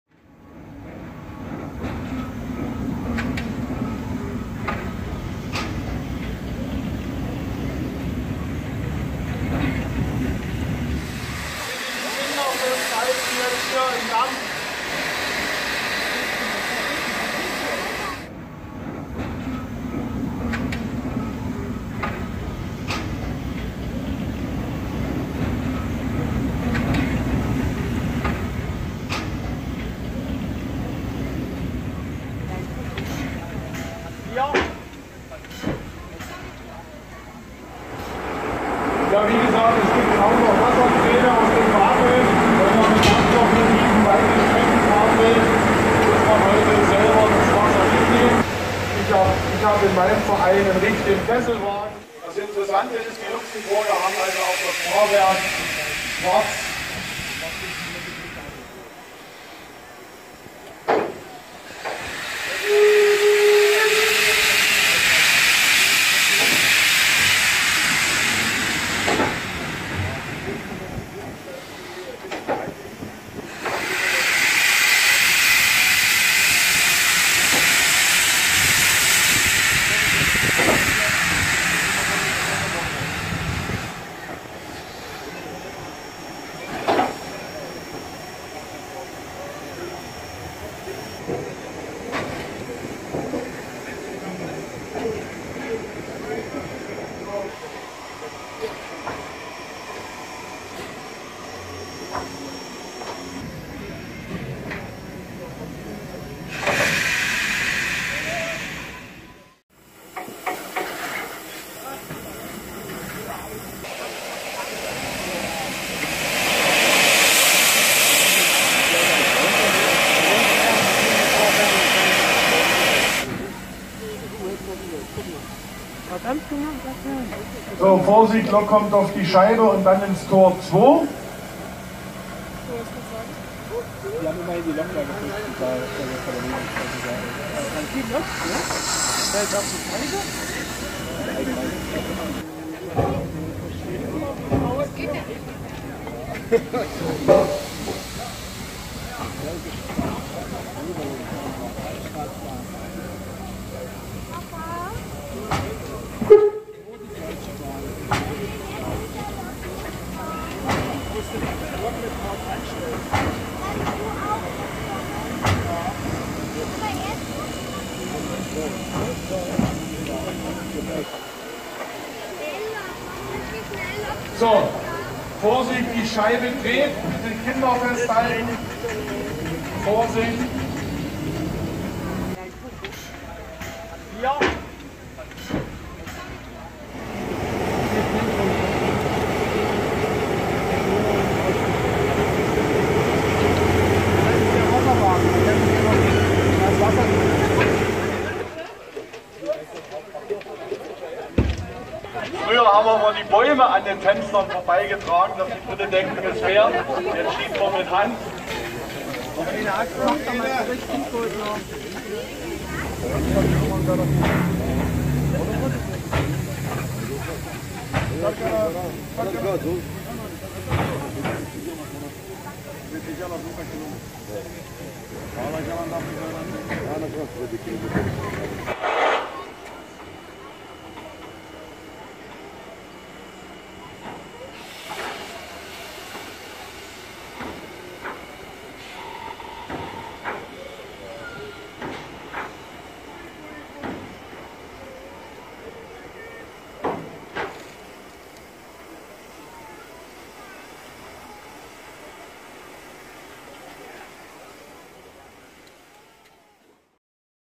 Dampflokfest Dresden 2017 - Eisenbahnfest // Dampfloks & Dieselloks der DR / DB - Drehscheibe
Hier ein paar bewegte Impressionen vom Dampflokfest in Dresden 2017 bei herrlichstem Frühlingswetter.